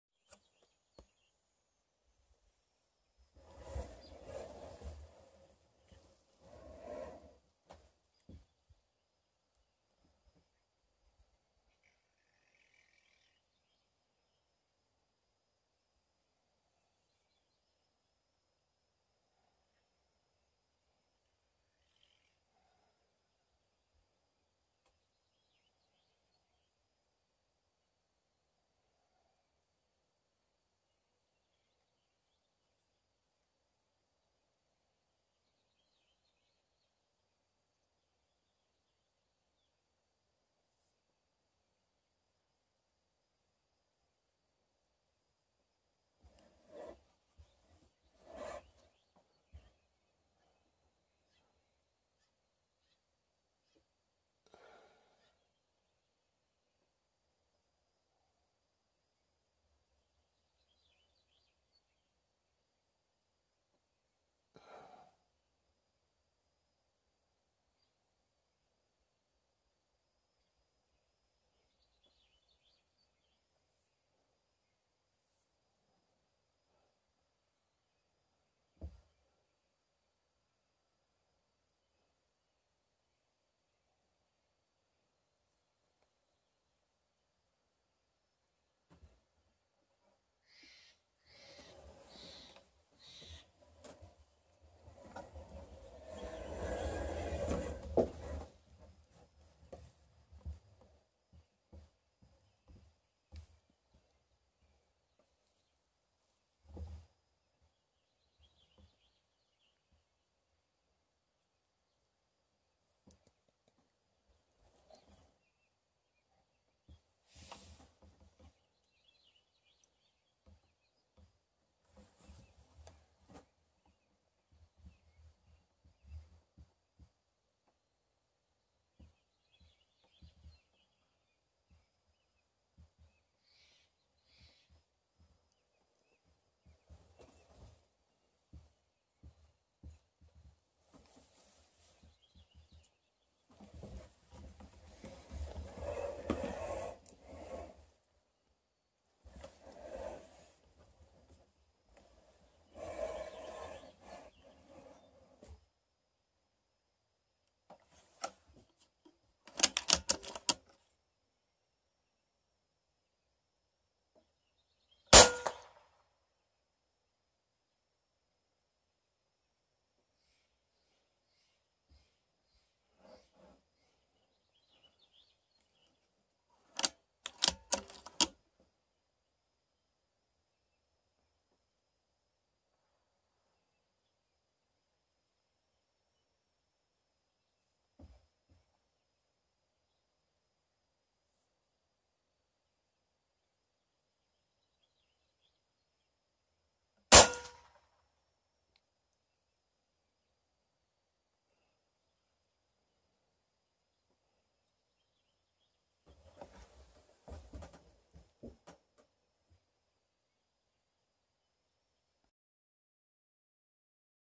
🎯xisico sentry hunting🎯 Prairie dogs
🔴 warning pesting video 🔴
 xisico sentry hunting prairie dogs three minutes of stalking and a mostly Silent Video.